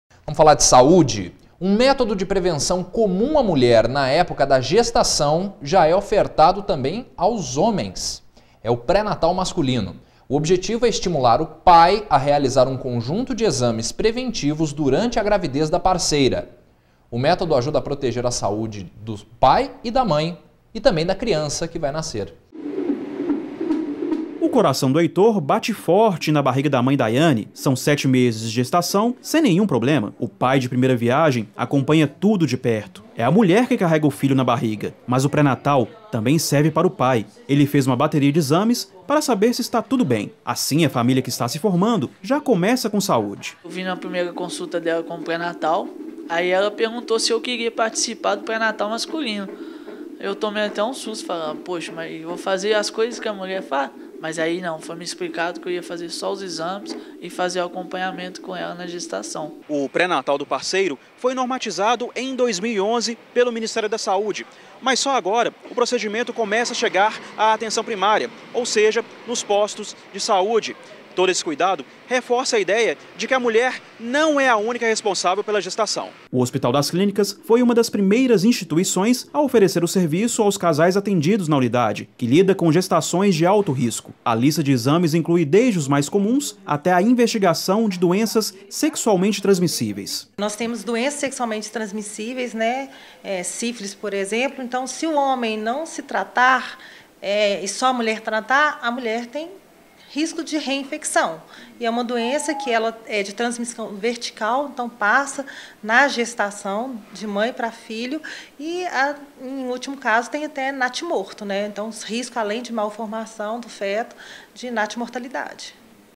Pré-natal masculino, bateria de exames para os pais pode prevenir doenças para a criança
Um método de prevenção comum à mulher na época da gestação já é ofertado também aos homens é o pré-natal masculino. O objetivo é estimular o pai a realizar um conjunto de exames preventivos durante a gravidez da parceira. O método ajuda a proteger a saúde dos pais e da criança que vai nascer.